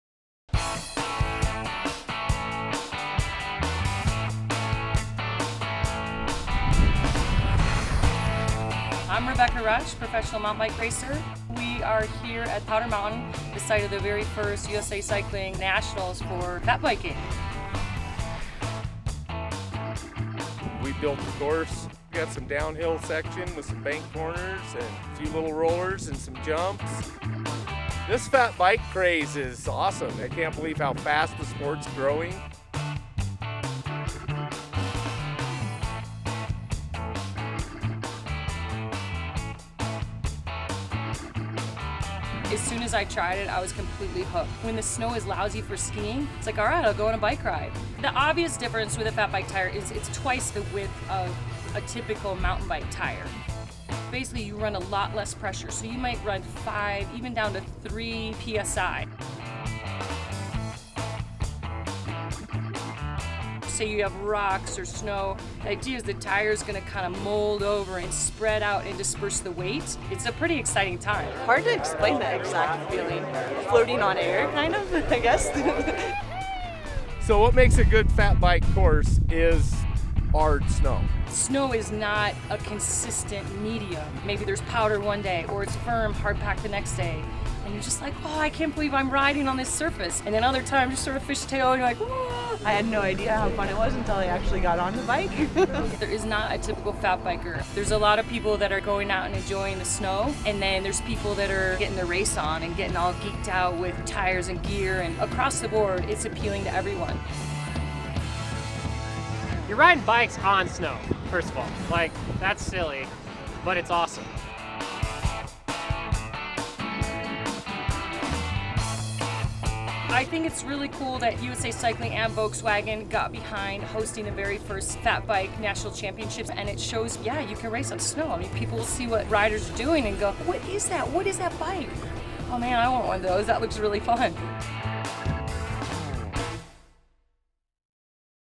2015 USA Cycling Fat Bike National Championships with Volkswagen
What is a Fat Bike? We checked it out this past weekend at the USA Cycling Fat Bike National Championships, up at Powder Mountain, UT. ‪#‎VW4Cycling‬